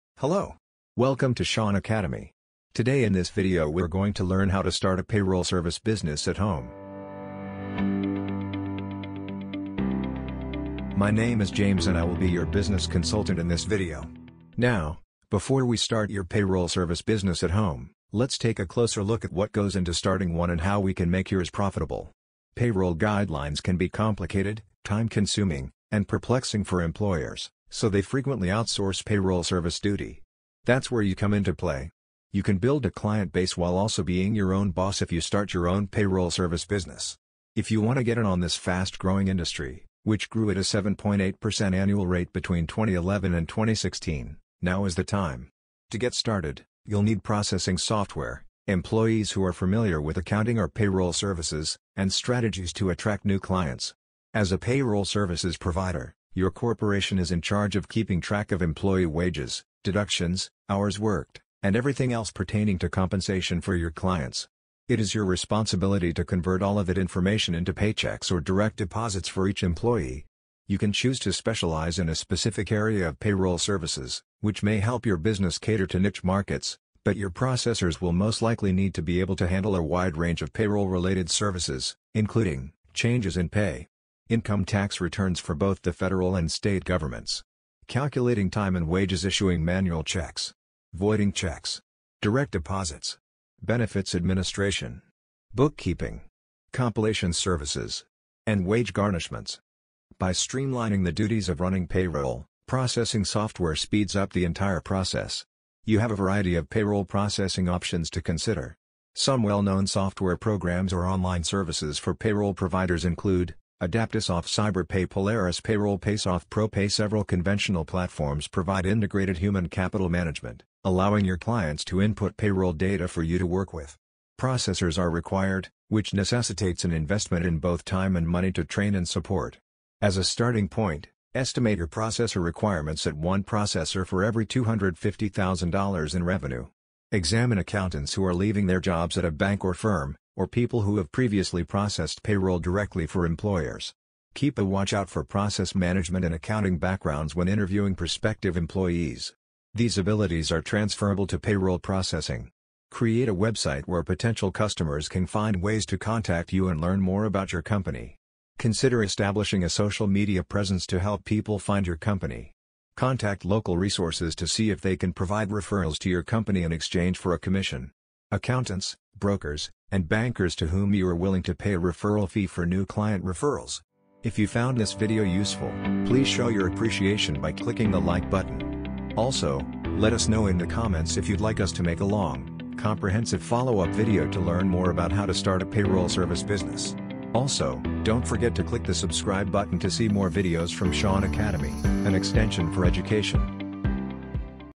How to Start a Payroll Service Business at Home | Grow Your Payroll Company Fast for Small Business
In this video, you will learn how to start a payroll service business at home. Let's take a closer look at what goes into starting a payroll service business at home and how we can make yours profitable before you get started.

Payroll regulations can be complex, time-consuming, and perplexing for employers, so they frequently outsource payroll service responsibility. This is where you come in. If you start your own payroll service business, you can build a client base while also being your own boss.

If you want to be a part of this rapidly expanding industry, which grew at a 7.8 percent annual rate between 2011 and 2016, now is the time. You'll need processing software, employees who are familiar with accounting or payroll services, and marketing strategies to get started.

This video will help you grow your payroll company fast to service a small business.